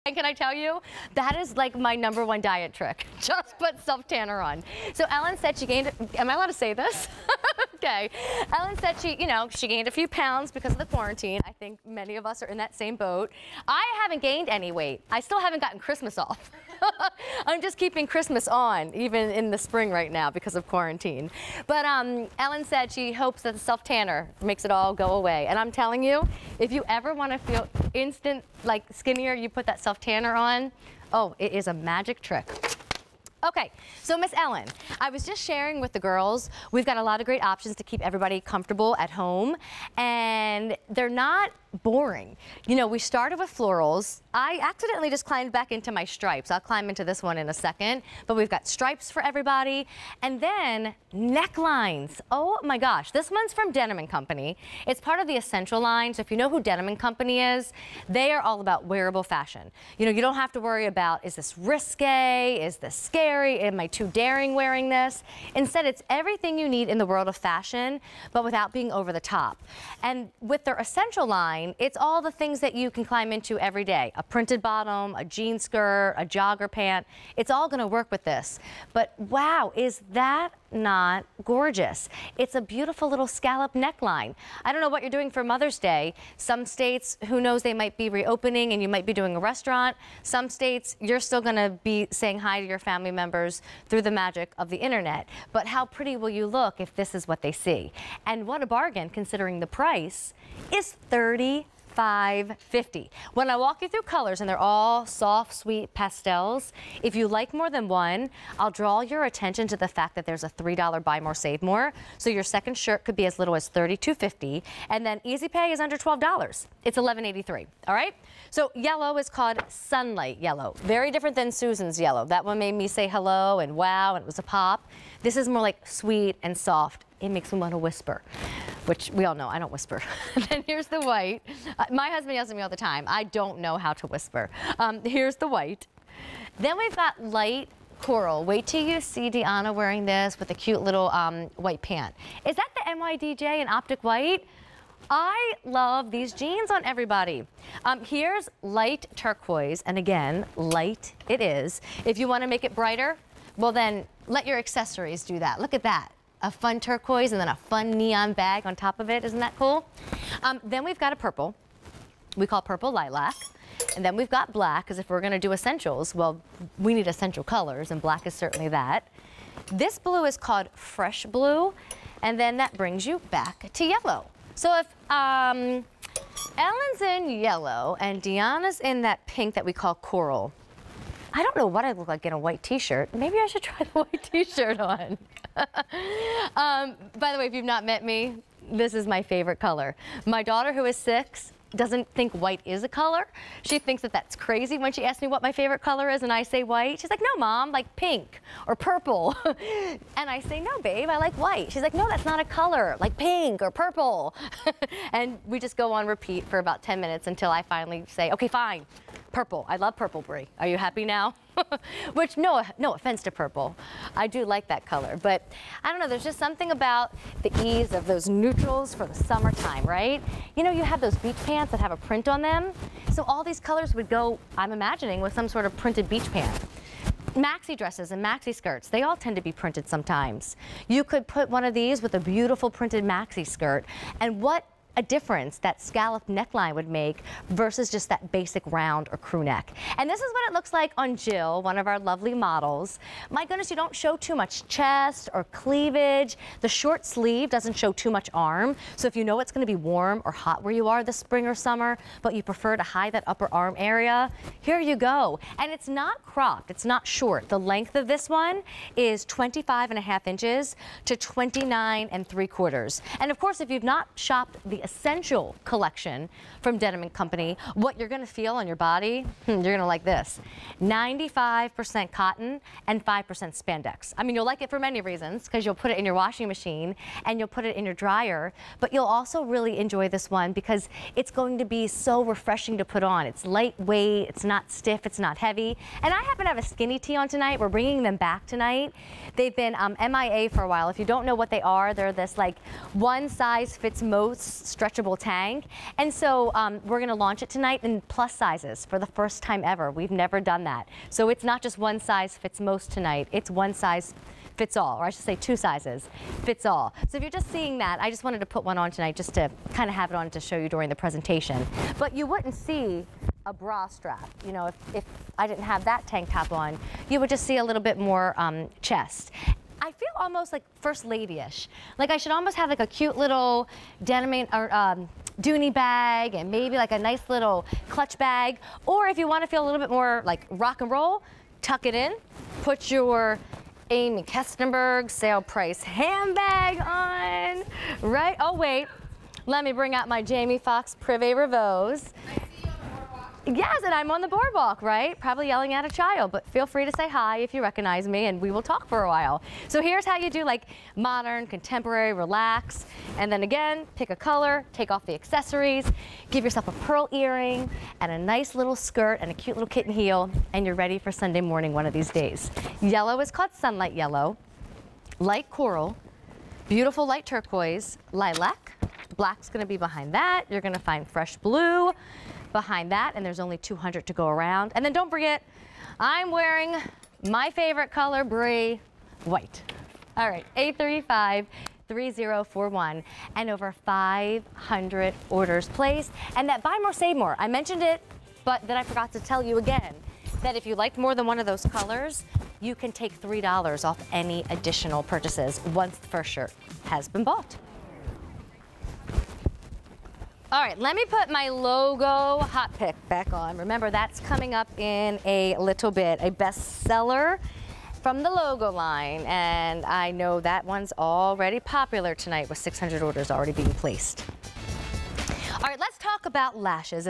Denim & Co. Essentials Perfect Jersey Top w/ Scallop Neckline on QVC
Denim & Co. Essentials Perfect Jersey Top w/ Scallop Neckline

Look ladylike in this timeless creation featuring a scalloped neckline. It will go perfectly with a pair of cigarette pants and ballet flats. From Denim & Co.&reg; Fashions.